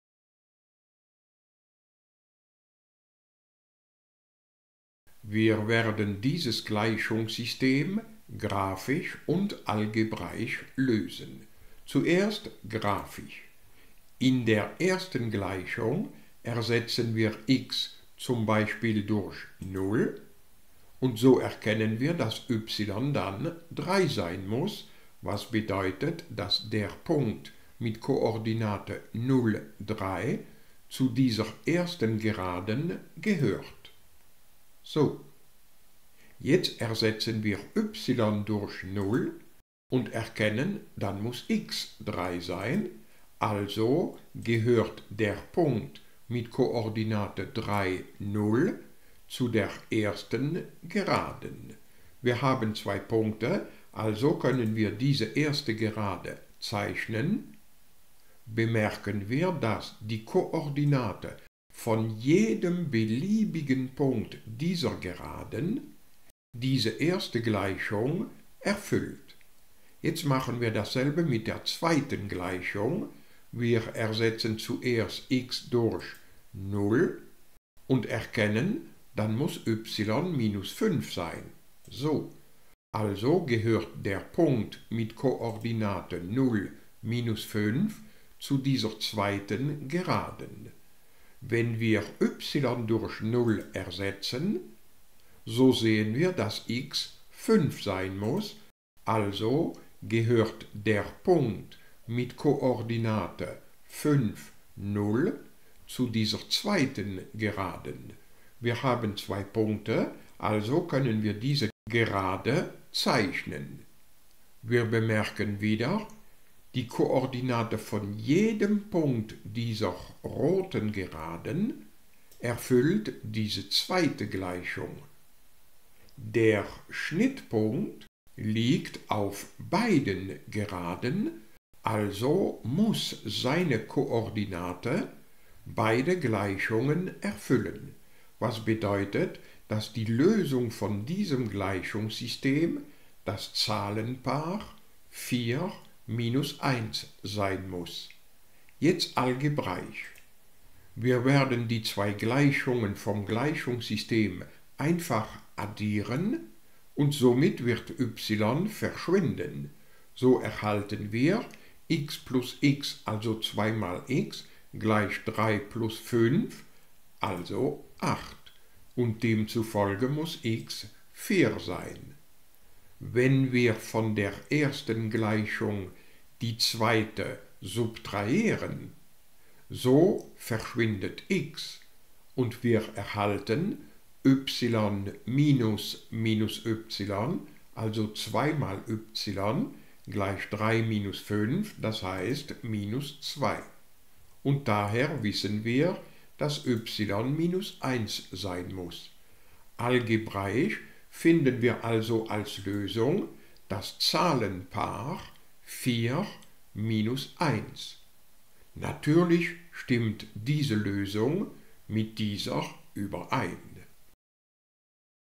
Gleichungssysteme D3030a
D3030a Wir lösen ein einfaches Gleichungssystem mit zwei Gleichungen und zwei Unbekannten graphisch und algebraisch.